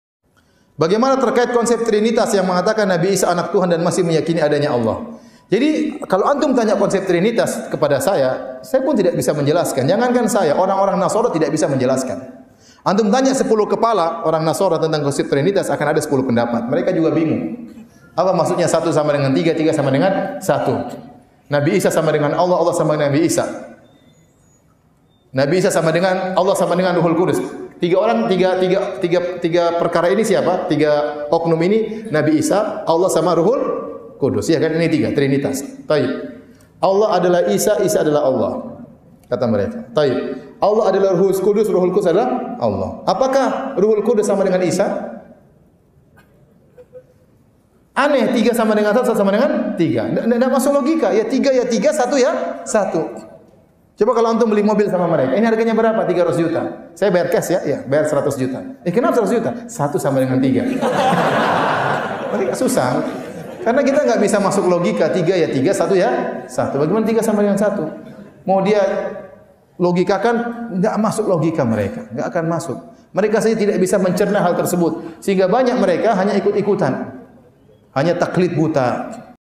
Bagaimana Dengan Konsep Trinitas?
👤 Ustadz Dr. Firanda Andirja, Lc., MA
🎬 Kontroversi Nabi Isa
🎥 Channel Youtube Firanda Andirja